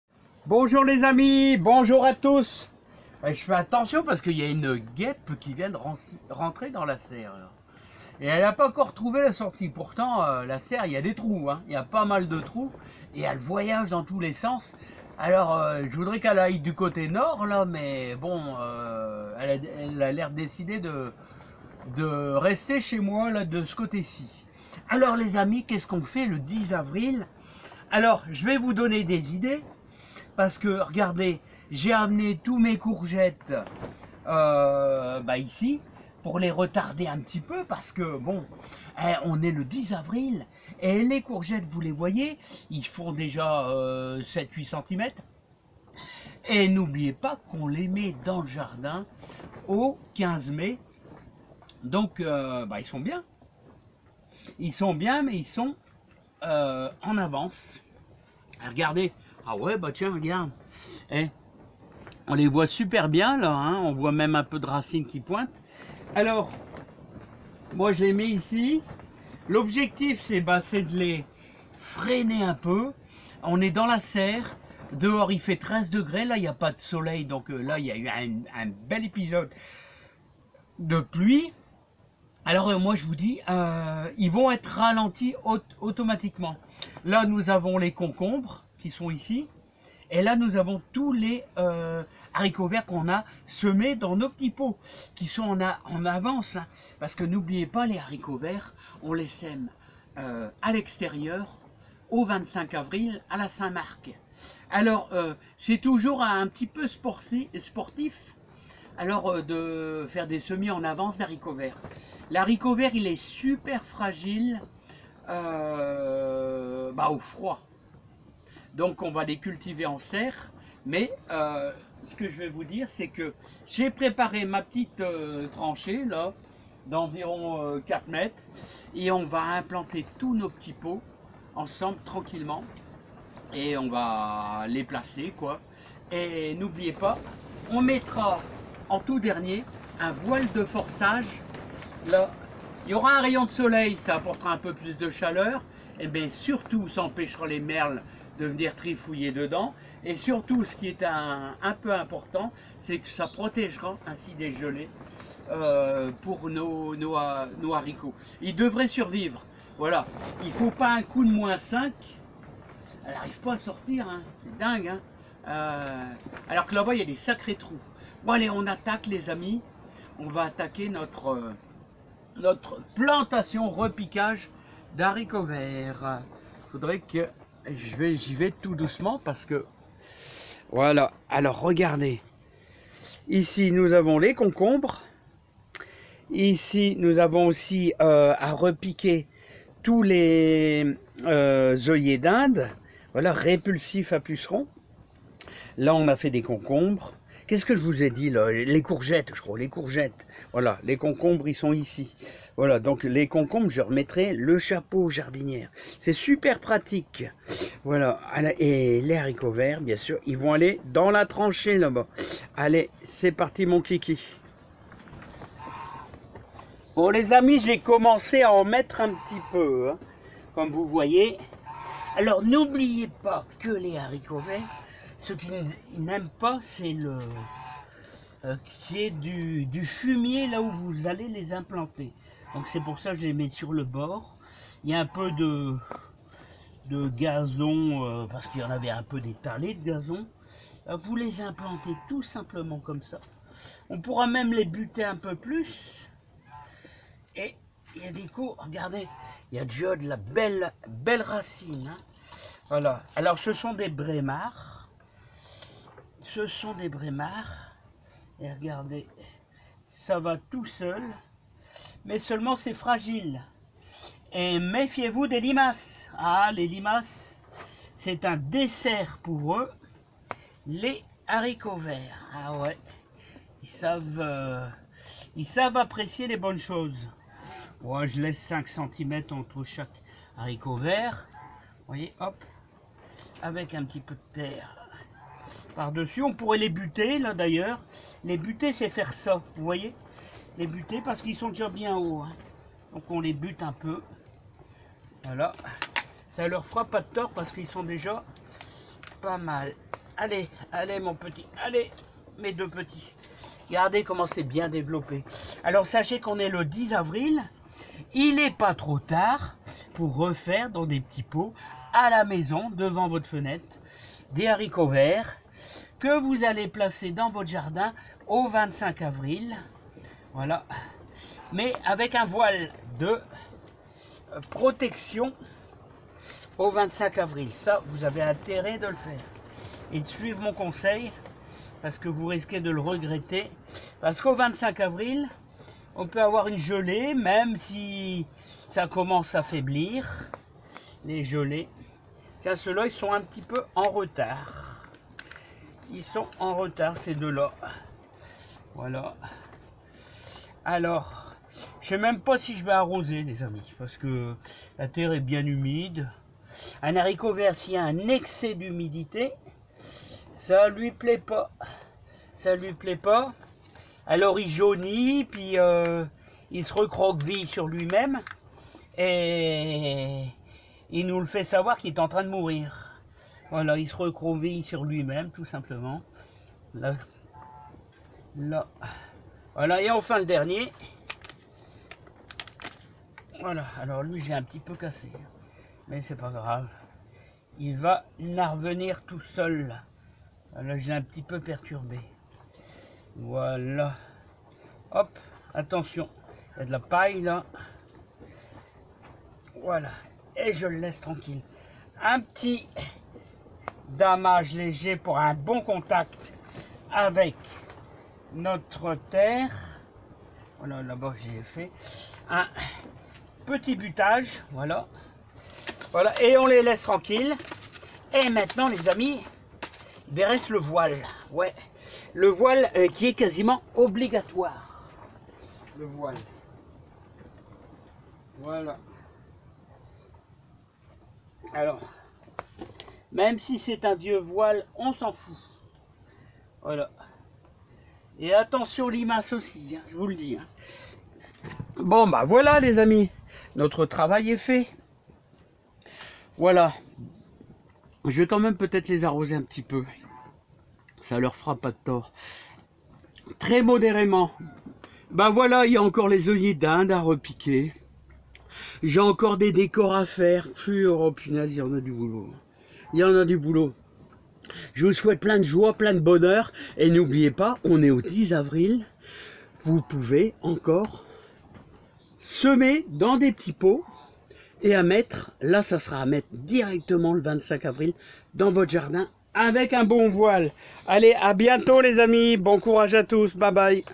10 avril au jardin ,haricots verts , concombres et meme courges a faire pour le potager
Pas trop tard pour semer dans des petits pots !